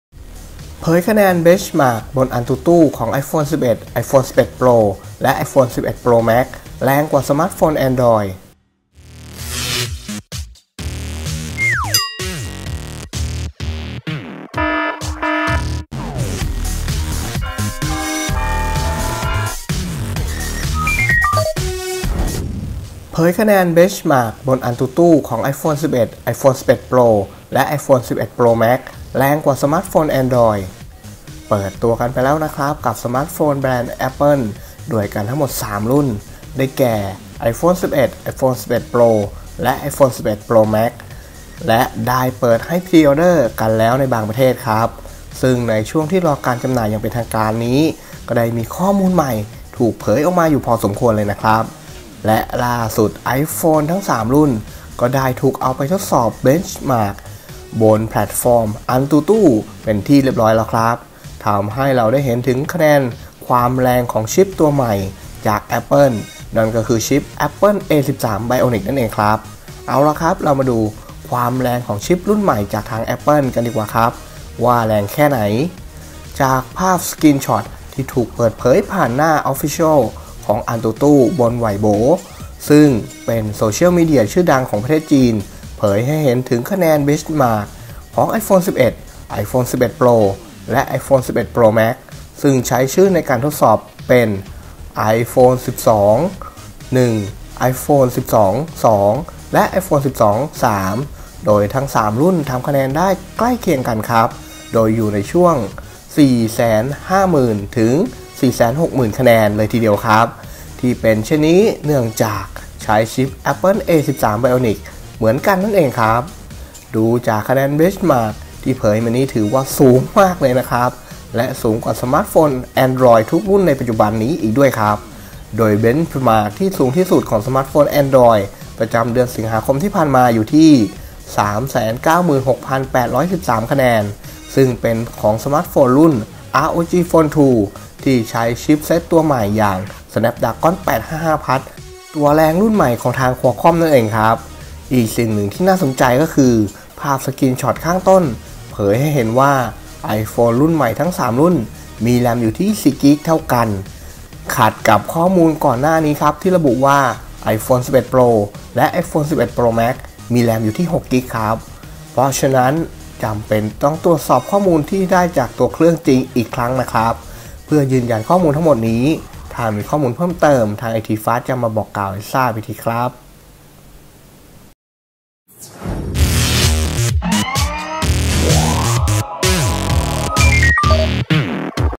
เผยคะแนน benchmark บน AnTuTu ของ iPhone 11 iPhone 11 Pro และ iPhone 11 Pro Max
910FFC77 68D3 4D74 B772 7C9BC65A78DB

เผยคะแนน benchmark บน AnTuTu ของ iPhone 11 iPhone 11 Pro และ iPhone 11 Pro Max แรงกว่าสมาร์ทโฟน Android แรงกว่าสมาร์ทโฟน Android
เปิดตัวกันไปแล้วนะครับกับสมาร์ทโฟนแบรนด์ Apple ด้วยกันสามรุ่นทั้ง  iPhone 11 iPhone 11 Pro และ iPhone 11 Pro Max ครับและได้เปิดให้ pre-order กันแล้วในบางประเทศ ซึ่งในช่วงที่รอการจำหน่ายอย่างเป็นทางการก็ได้มีข้อมูลใหม่ถูกเผยออกมาอยู่พอสมควรเลยนะครับ
และล่าสุด iPhone ทั้ง 3 รุ่นนี้ก็ได้ถูกเอาไปทดสอบ benchmark บนแพลตฟอร์ม AnTuTu เป็นที่เรียบร้อยแล้วครับ ทำให้เราได้เห็นความแรงของชิปเซ็ตตัวใหม่จากทาง Apple นั่นก็คือชิป Apple A13 Bionic นั่นเองครับ
เอาละครับเรามาดูความแรงของชิปรุ่นใหม่จากทาง Apple กันดีกว่าครับว่าจะแรงแค่ใหน่
จากภาพสกรีนช็อตที่ถูกเปิดเผยผ่านหน้า official ของ AnTuTu บน Weibo ซึ่งเป็นโซเชียลมีเดียชื่อดังของประเทศจีน เผยให้เห็นคะแนน benchmark ของ iPhone 11, iPhone 11 Pro และ iPhone 11 Pro Max ซึ่งใช้ชื่อในการทดสอบเป็น iPhone 12, 1 iPhone 12, 2 และ iPhone 12, 3 โดยทั้ง 3 รุ่นทำคะแนนได้ใกล้เคียงกันโดยอยู่ในช่วง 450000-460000 คะแนนเลยทีเดียวครับ ที่เป็นเช่นนี้เนื่องจากใช้ชิปเซ็ต Apple A13 Bionic เหมือนกันนั่นเองครับ
ดูจากคะแนน benchmark ที่เผยมานี้ถือว่าสูงมากเลยนะครับและสูงกว่าสมาร์ทโฟน Android ทุกรุ่นในปัจจุบันนี้อีกด้วยครับ โดย benchmark ที่สูงที่สุดของสมาร์ทโฟน Android ประจำเดือนสิงหาคมที่ผ่านมา อยู่ที่ 396813 คะแนน ซึ่งเป็นของสมาร์ทโฟนรุ่น ROG Phone 2 ที่ใช้ชิปเซ็ต Snapdragon 855+ ตัวแรงรุ่นใหม่ของทาง Qualcomm นั่นเองครับ
อีกสิ่งหนึ่งที่น่าสนใจ ภาพสกรีนช็อตข้างต้นเผยให้เห็นว่า iPhone รุ่นใหม่ทั้ง 3 รุ่น มี RAM 4GB เท่ากัน ขัดกับข้อมูลก่อนหน้านี้ที่ระบุว่า iPhone 11 Pro และ iPhone 11 Pro Max มี RAM 6GB ครับ
เพราะฉนั้นจำเป็นต้องตรวจสอบข้อมูลที่ได้จากตัวเครื่องจริงอีกครั้งนะครับ เพื่อยืนยันข้อมูลทั้งหมดนี้ครับ
ถ้ามีข้อมูลเพิ่มเติมทาง IT FAST จะมาบอกกล่าวอีกทีนะครับ